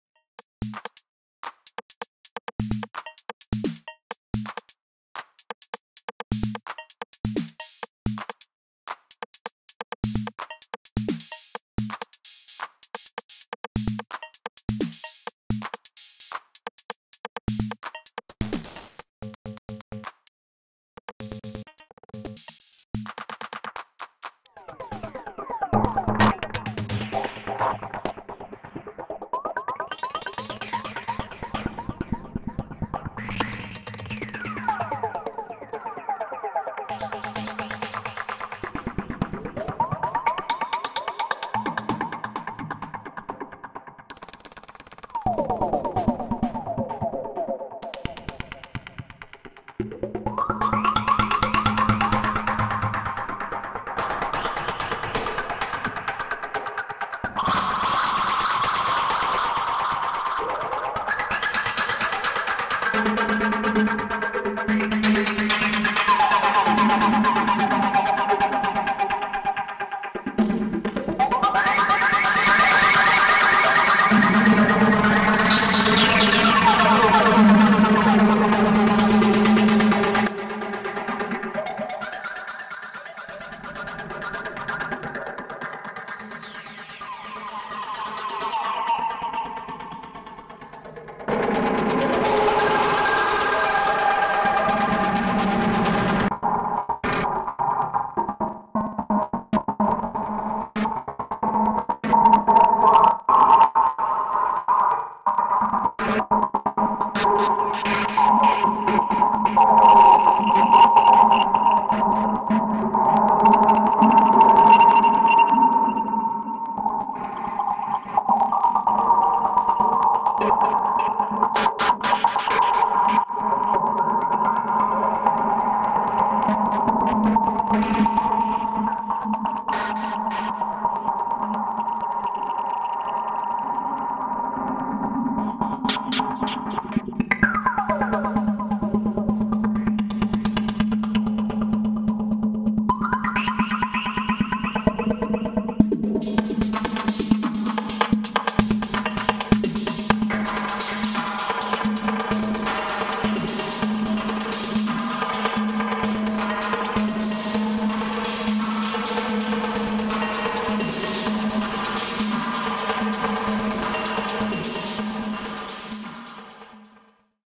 Too Much Audio Damage
Audio Damage just released all their legacy plug-ins for free - 33 in total. Here's 19 of them loaded on a single track instance for absolutely no good reason - on the *least* compatible scenario, even, an Apple Silicon Mac running the latest OS in a host with no bridges running natively.